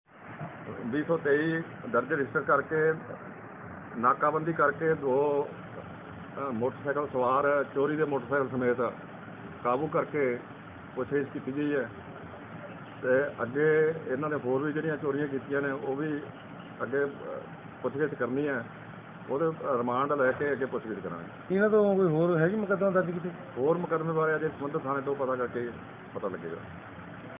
ਪਟਿਆਲੇ ਵਿਖੇ ਸ਼ੱਕ ਦੇ ਆਧਾਰ ਤੇ ਰੋਕਿਆਂ ਵਿਅਕਤੀ ਕੋਲੋਂ ਨਿਕਲਿਆ ਚੋਰੀ ਦਾ ਮੋਟਰਸਾਈਕਲ, ਅੱਗੇ ਹੋਰ ਜਾਂਚ ਹੋਣੀ ਬਾਕੀ ਹੈ
ਪਟਿਆਲੇ ਵਿਖੇ ਸ਼ੱਕ ਦੇ ਆਧਾਰ ਤੇ ਰੋਕਿਆਂ ਵਿਅਕਤੀ ਕੋਲੋਂ ਨਿਕਲਿਆ ਚੋਰੀ ਦਾ ਮੋਟਰਸਾਈਕਲ